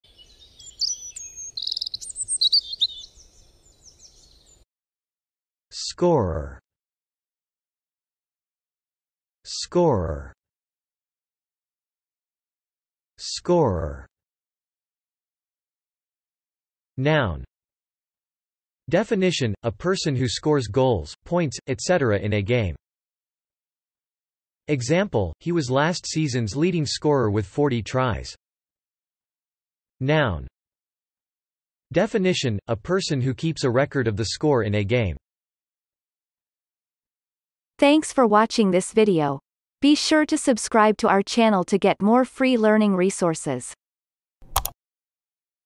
How to pronounce scorer - Vocab Today
The pronunciation of the word scorer in Amercian accent is demonstrated in this video. If the word is from another language, such as brand name, it will be pronounced in its original language. Based on the word frequency, this is of 3 stars difficulty.

The word scorer has the following meanings:
1. (noun)
Definition : A person who scores goals, points, etc. in a game.
Example : he was last season's leading scorer with 40 tries
2. (noun)
Definition : A person who keeps a record of the score in a game.